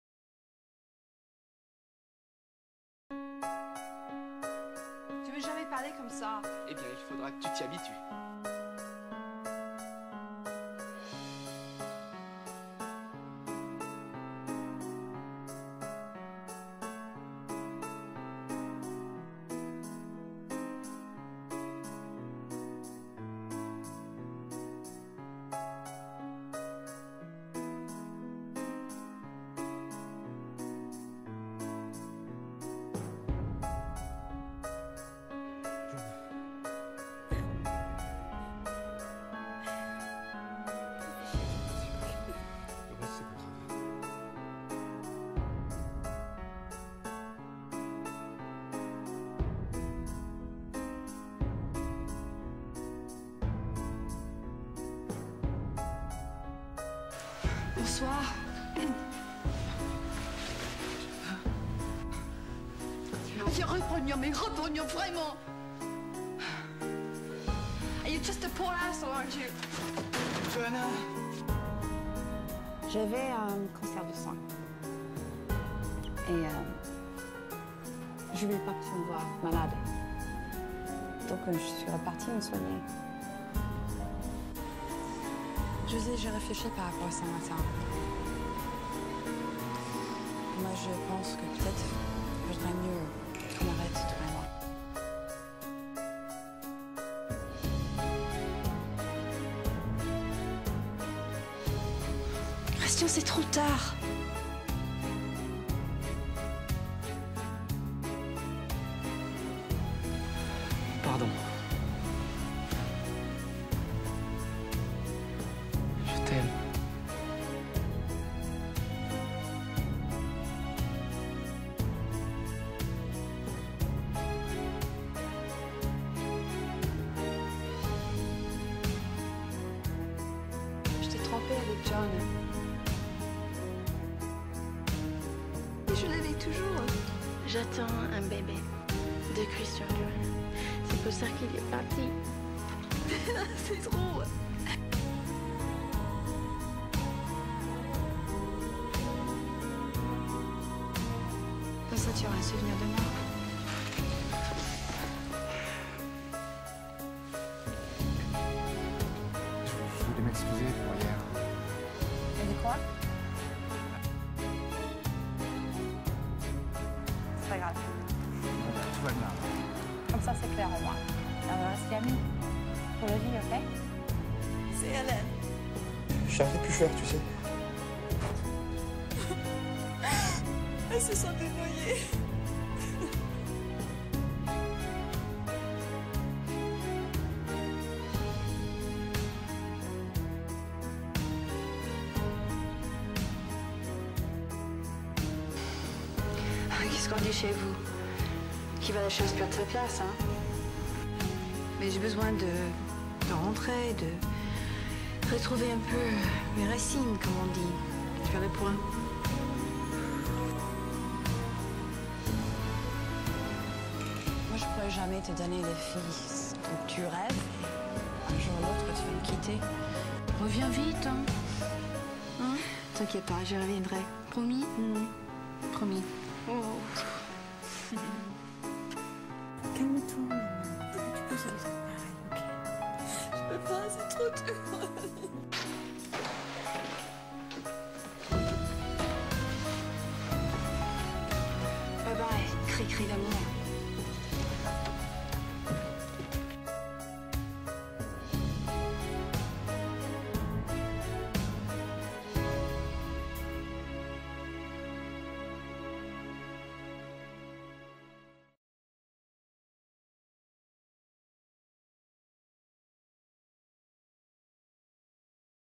Les peines de Johanna
Voici les épreuves douloureuse de la vie qu'a connue Johanna. Personnage magnifiquement bien interprété par Rochelle Redfield qui arrive à faire passer de vrais émotions.